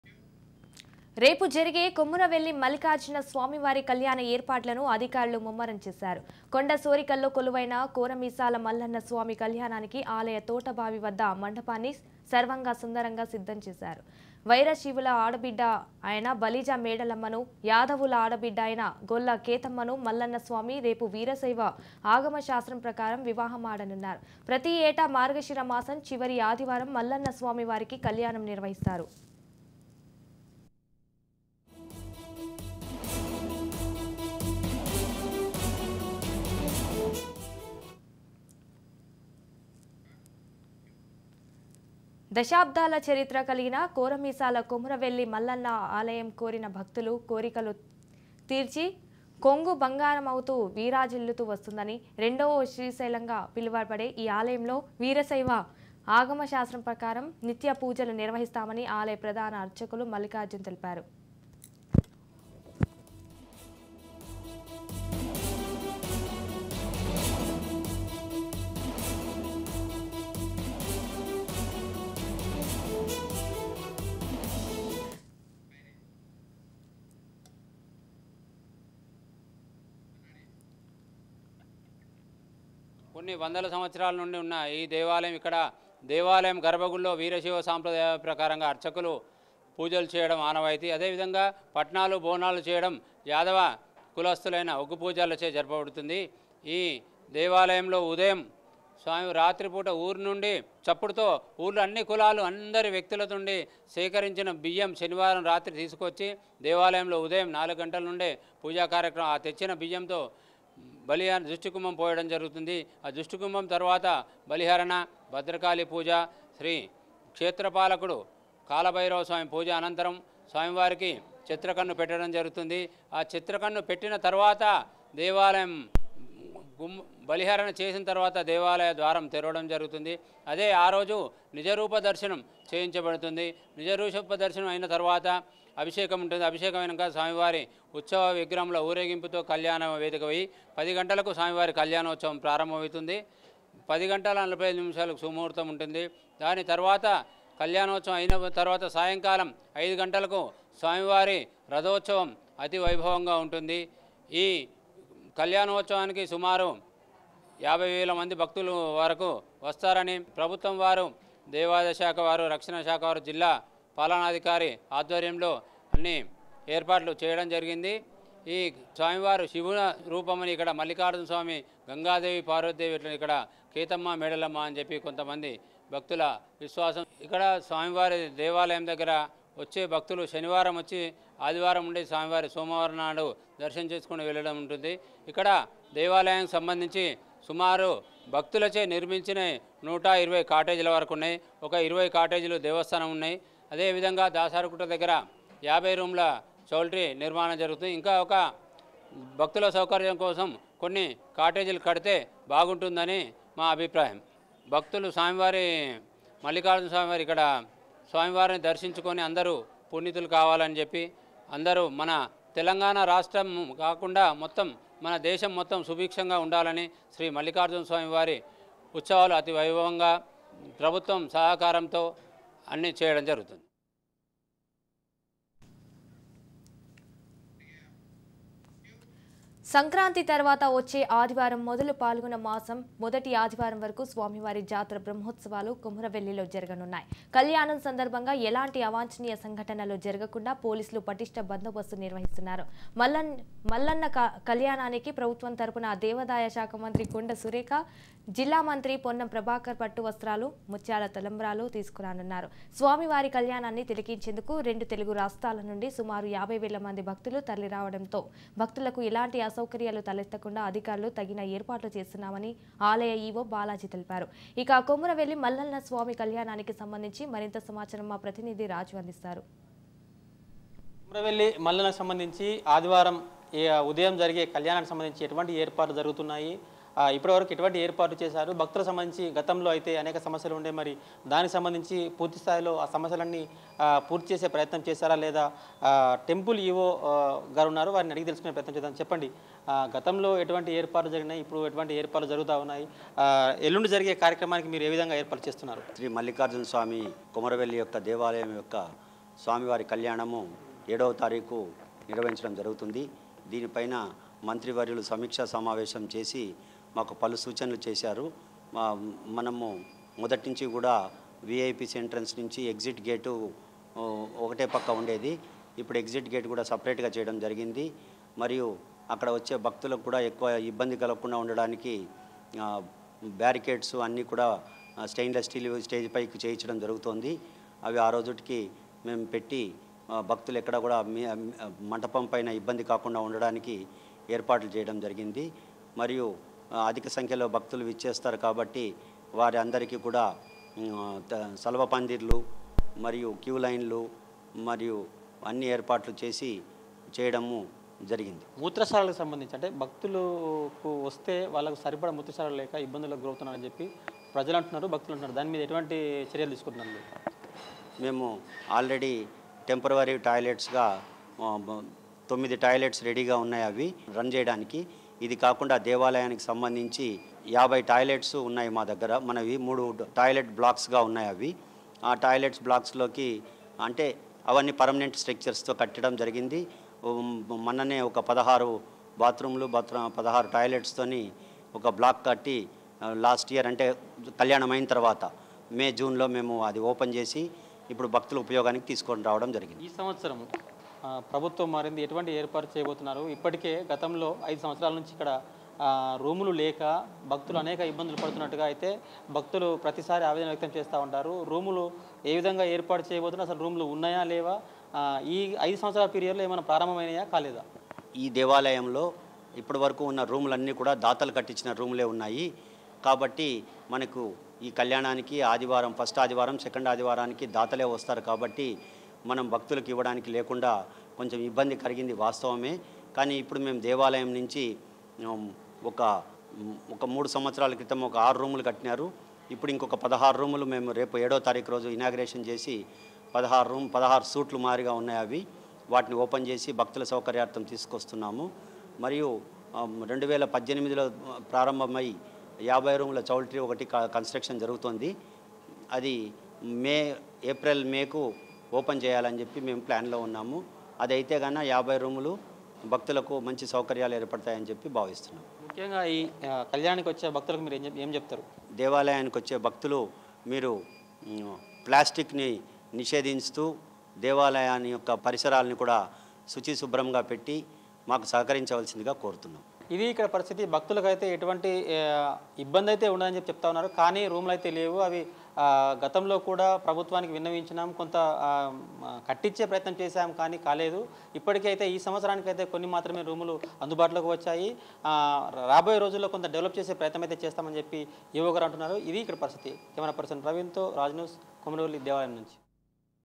Temple Management Set All Arrangements For Mallanna Kalyanam || Raj News Telugu
రేపు ఘనంగా కొమురవెల్లి మల్లన్న స్వామి కళ్యాణం || Temple Management Set All Arrangements For Mallanna Kalyanam || @diyakitchens